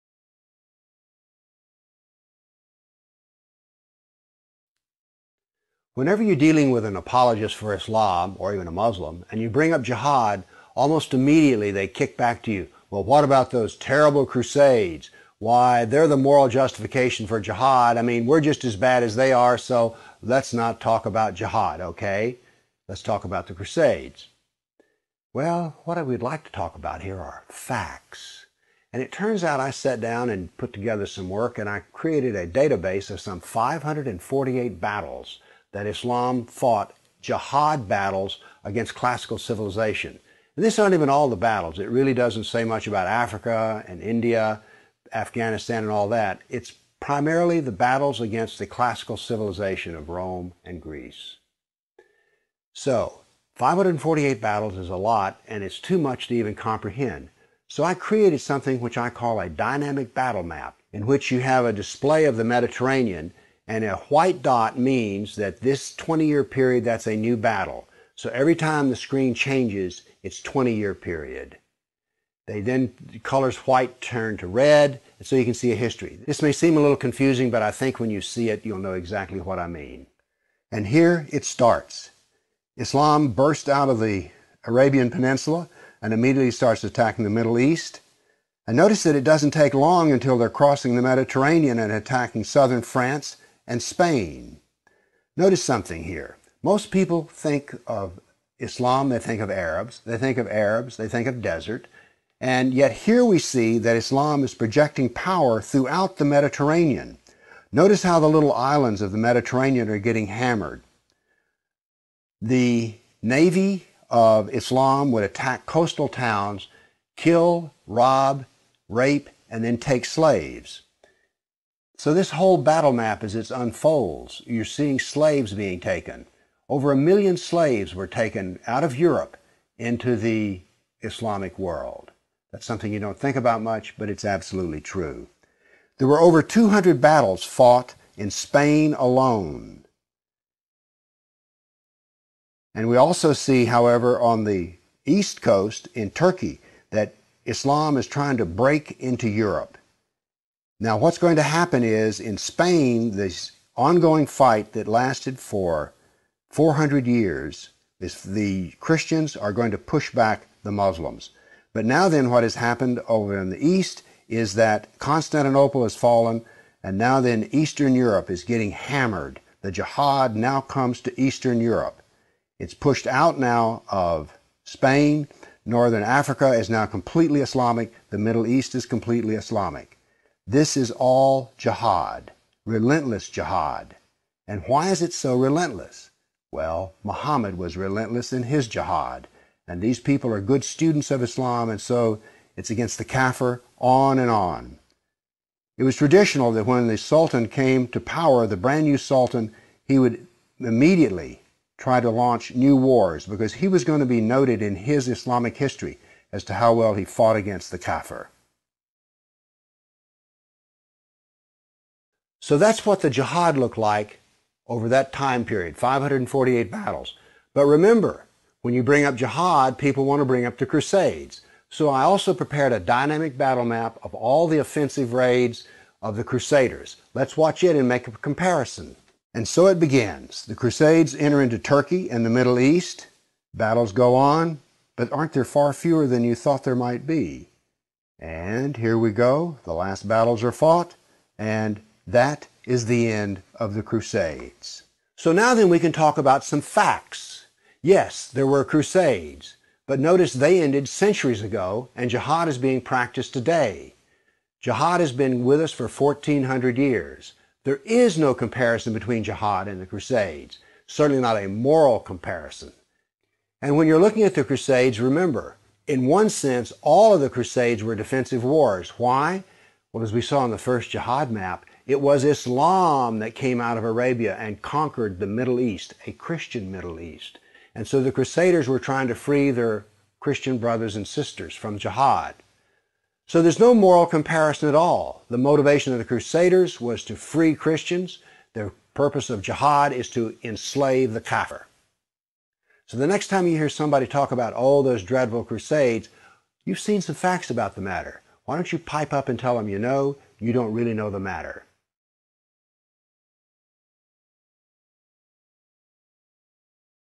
Muslim Jihad Enslaving Europe and Christian Middle East
Bill Warner Phd political Islam

common creative reuse allowed upload from youtube channel: TOMMY ROBINSON PATRIOT